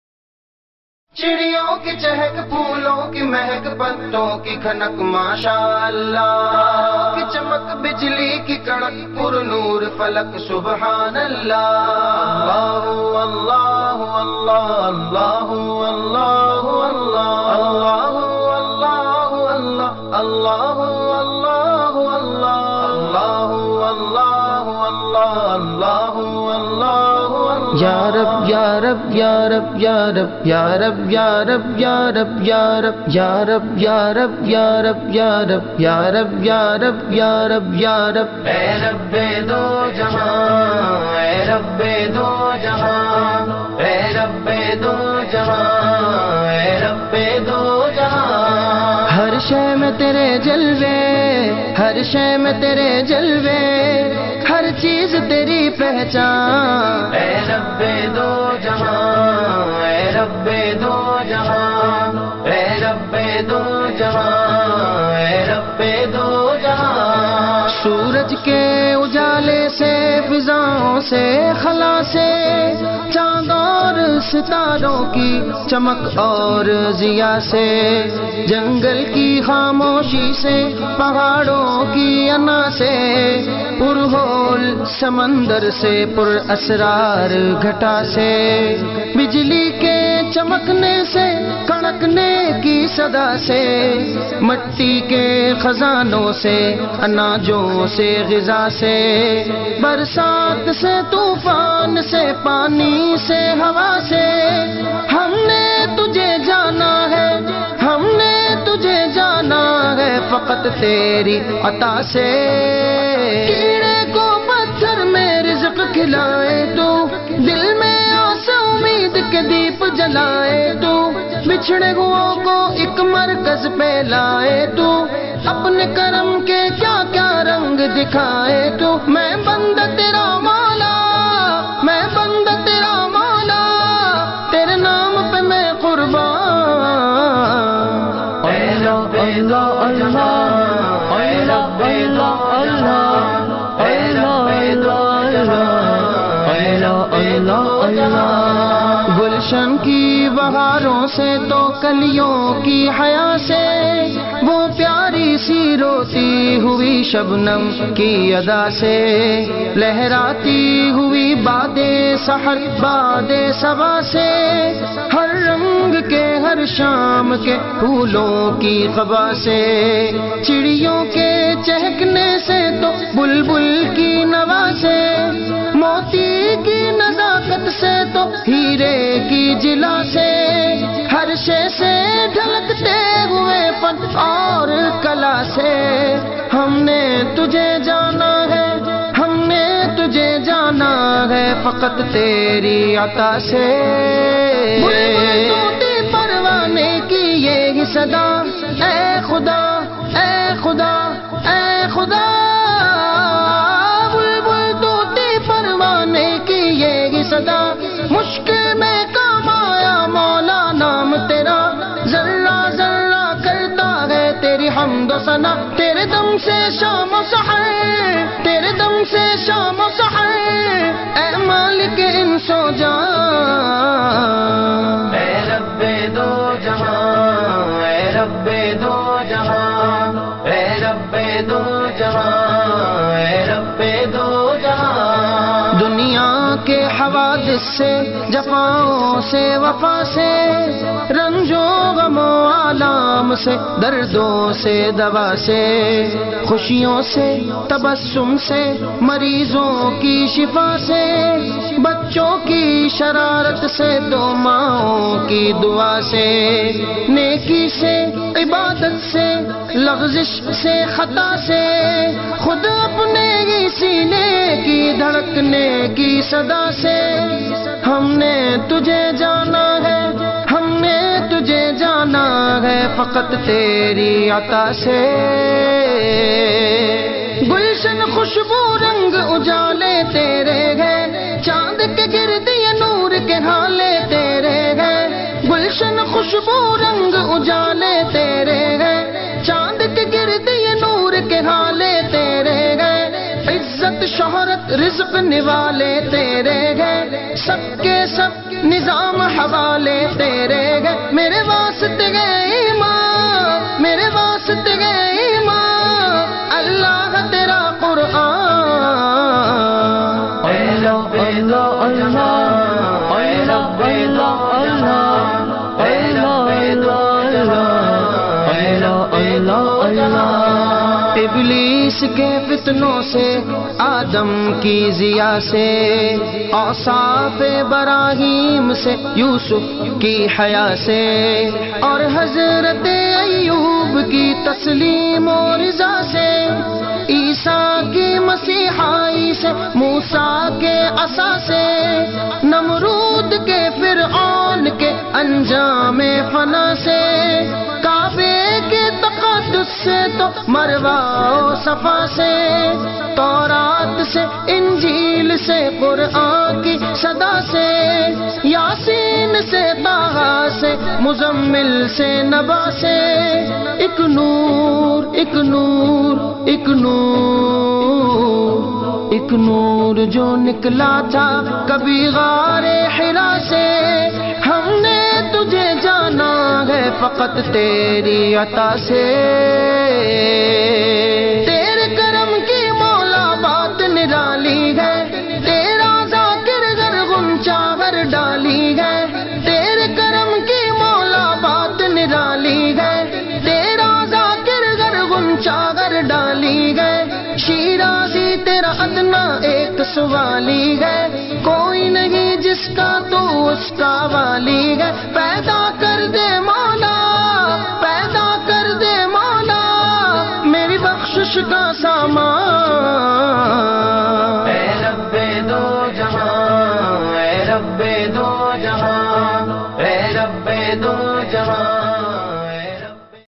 عرس و جلسہ دستارِفضیلت 2018
عرس مبارک و جلسہ دستار فضیلت
منجانب جامعہ علویہ غوثیہ 28 اکتوبر 2018